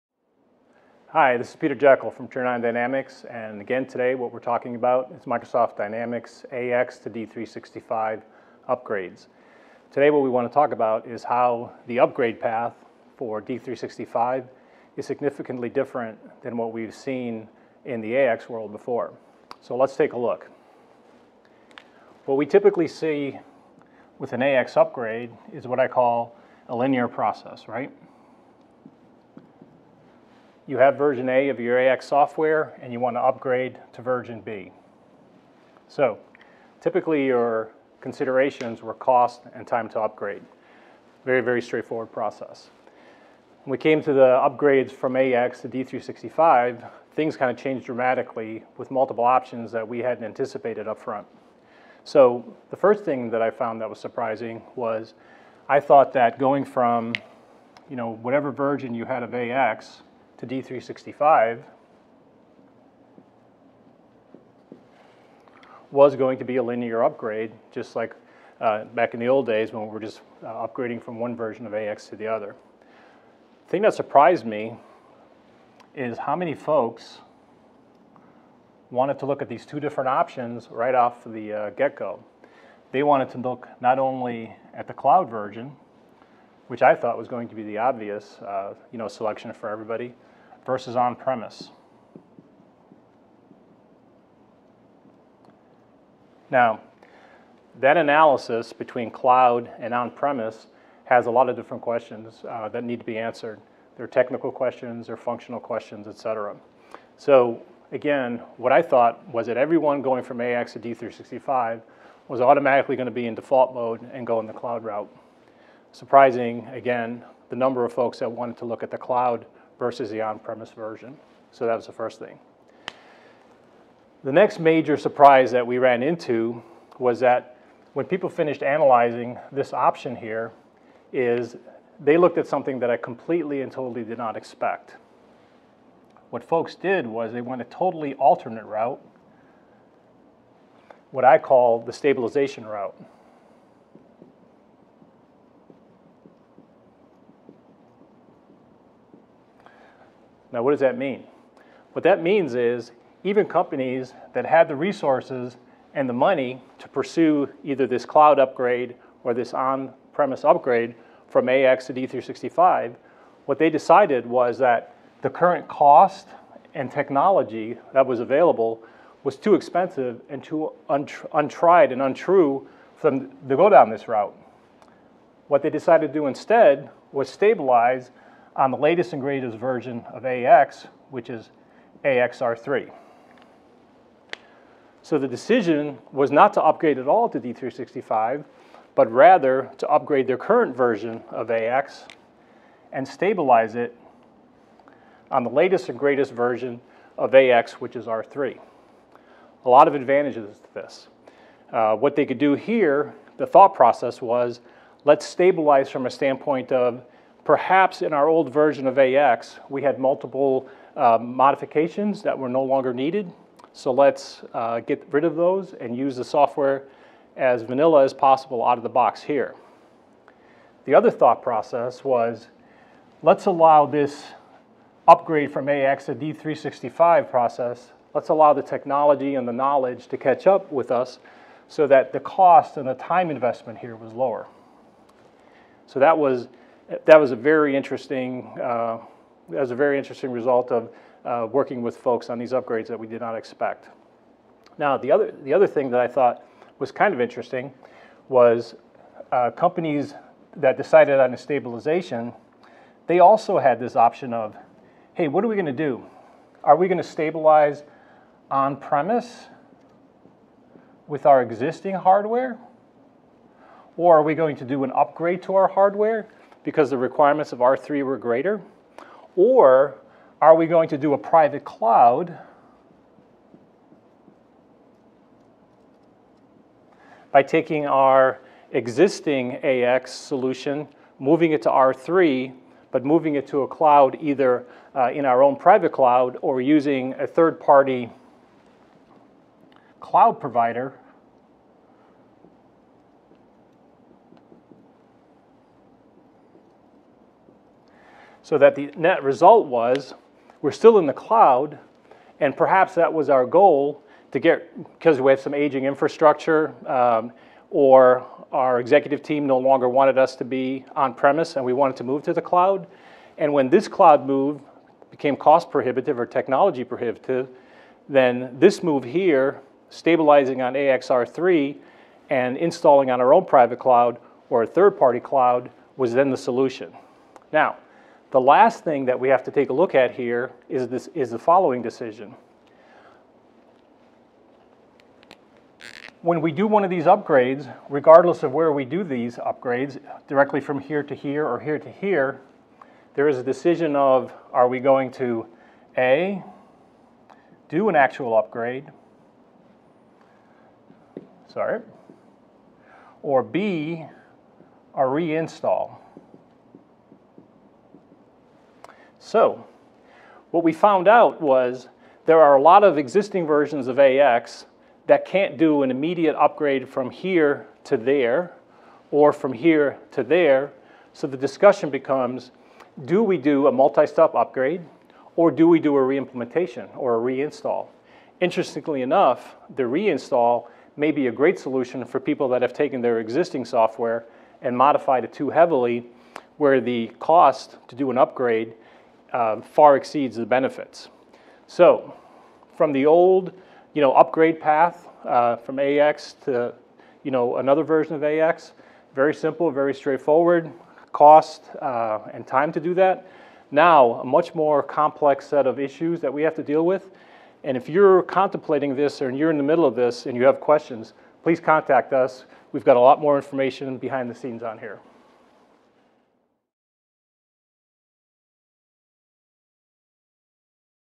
TurnOnDynamics - AX Upgrade Paths and Potholes
Microst Dynamics AX to Dynamics D365 for Finance and Operations upgrades are complex and expensive.

In addition, these upgrades don't follow a typical linear upgrade path. Learn how your upgrade project can be affected.